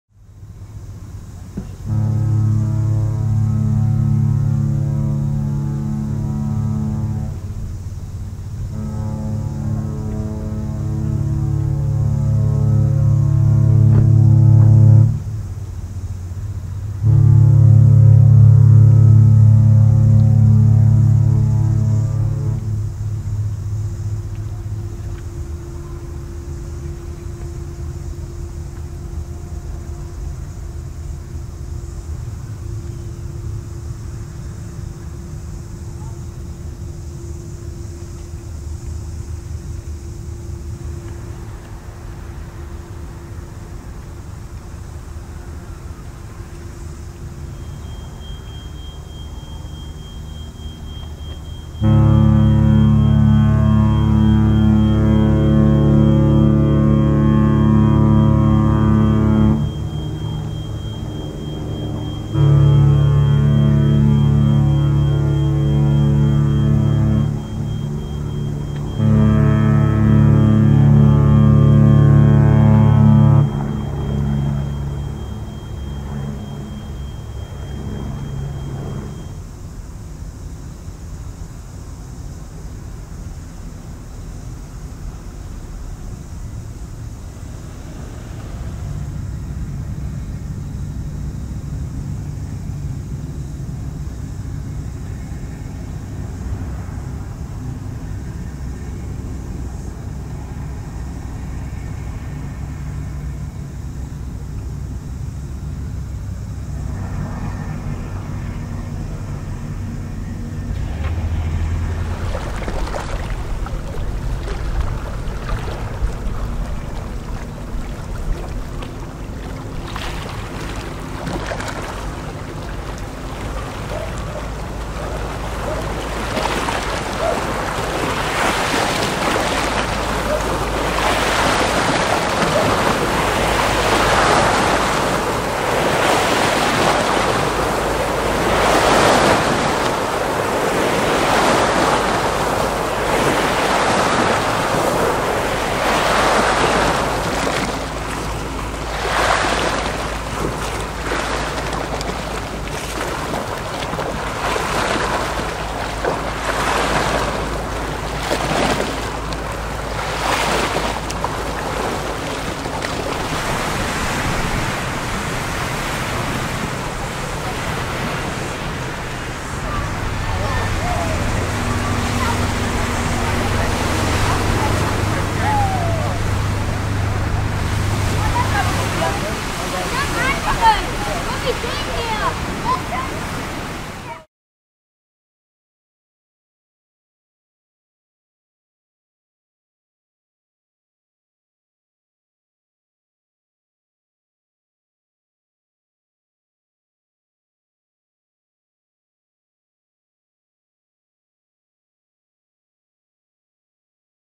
Celebrity Solstice - Passenger Ship
On the 17th of February 2018 the Passenger Ship Celebrity Solstice departed Auckland